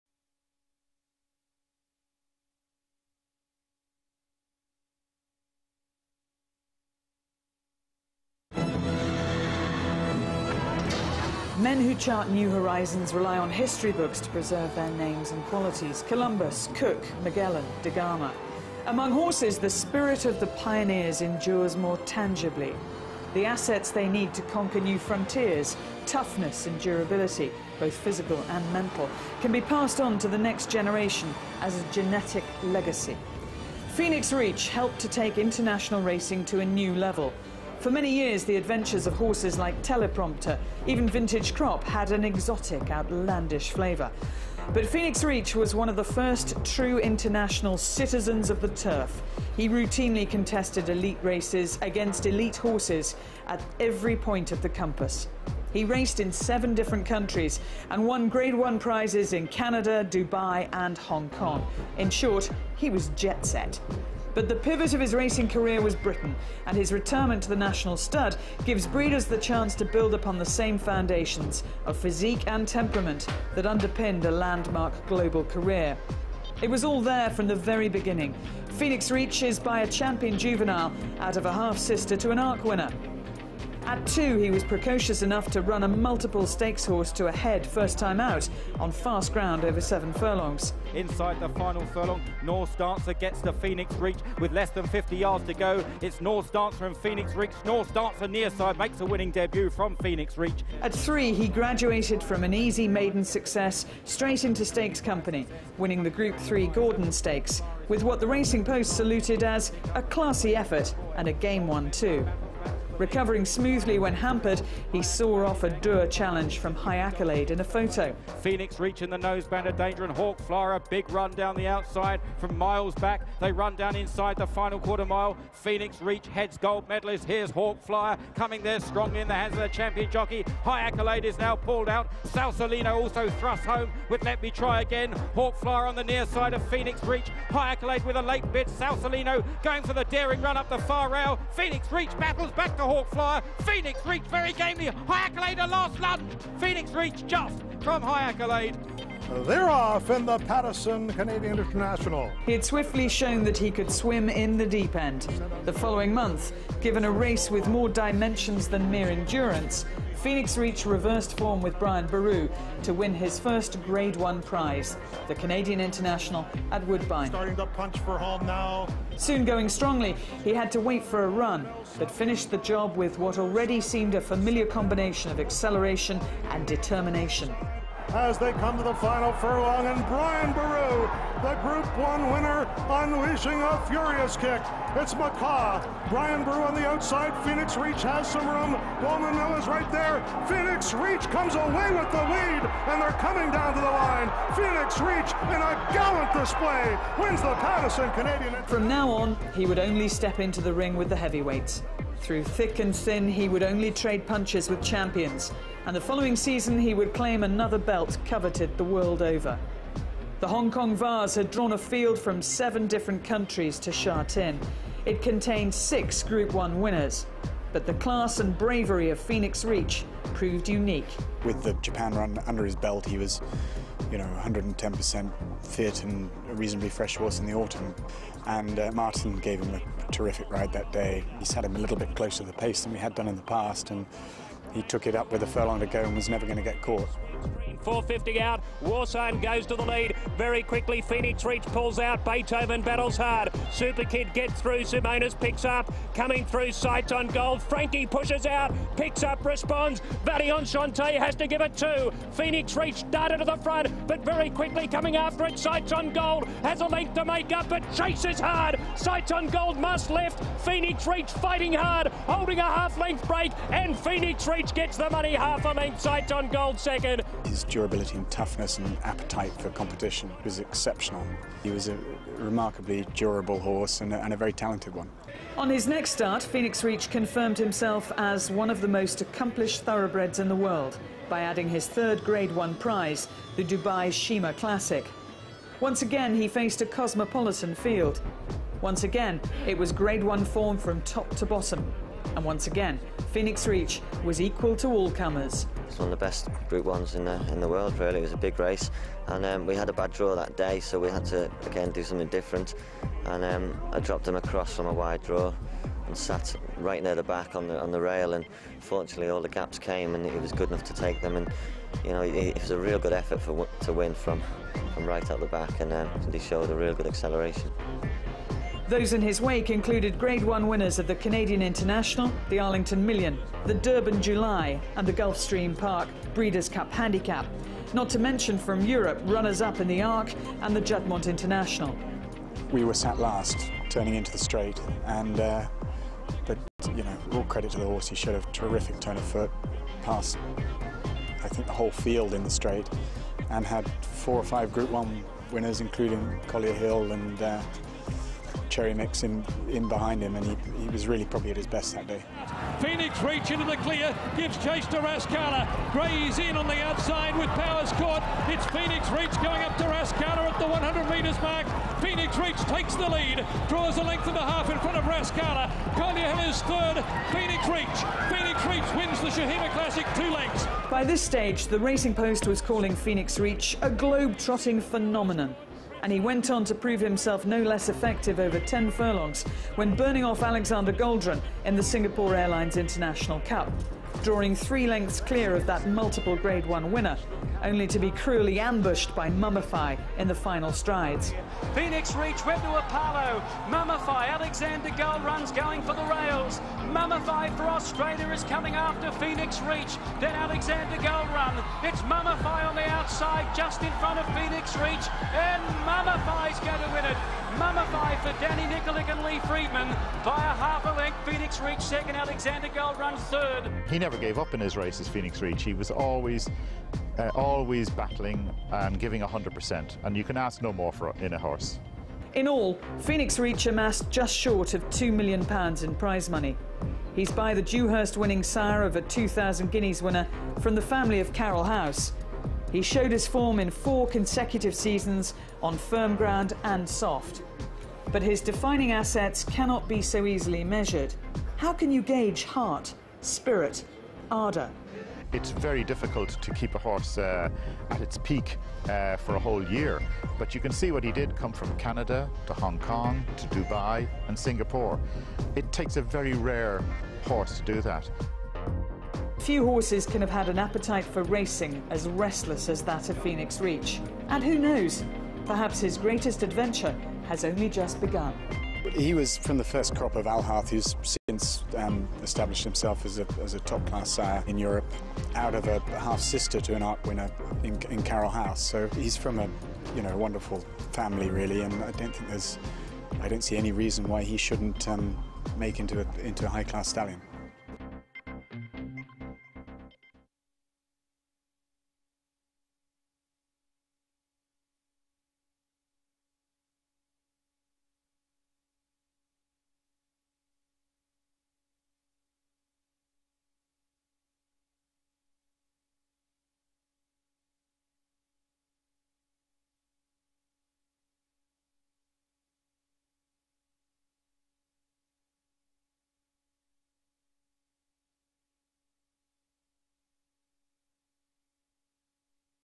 Phoenix Reach Presentation
Profile of Phoenix Reach, Globetrotting Triple Group 1 winning horse, retiring to stud in 2007.